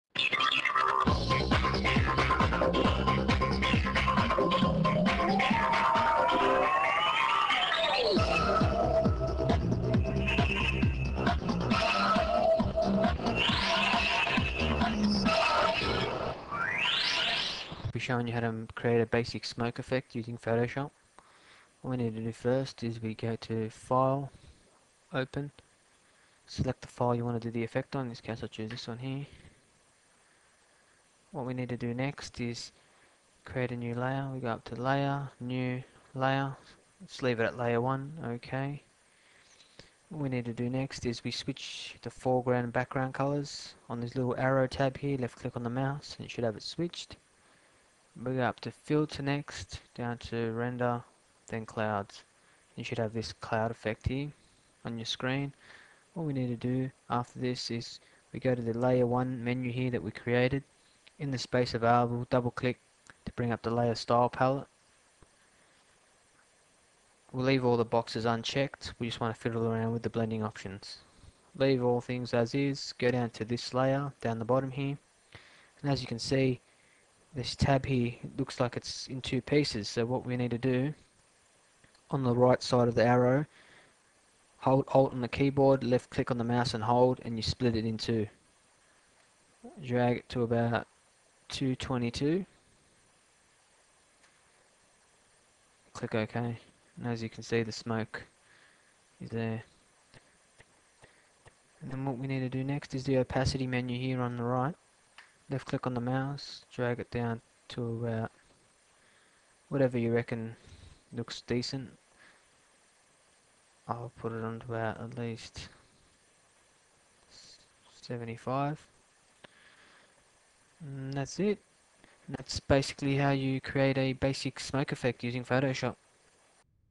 Smoke Effect Using Photoshop
In this free video tutorial provided by Luv2Help.com we will show you how to create a realistic looking smoke effect using Adobe Photoshop.
for high resolution viewing and more free video tutorials.